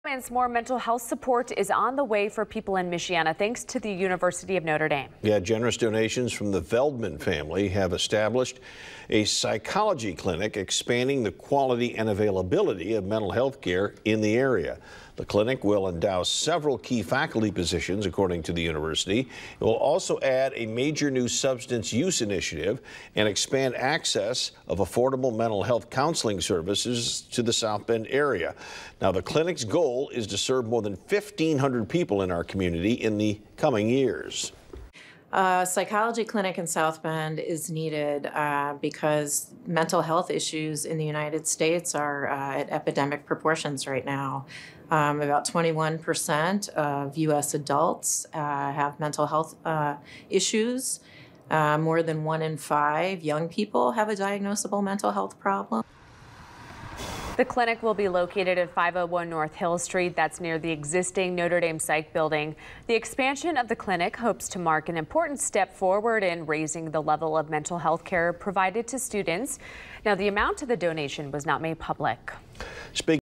Notre Dame to expand mental health services with new clinic
Generous donations from the Veldman family have helped establish a psychology clinic, expanding the quality and availability of mental health care in the area.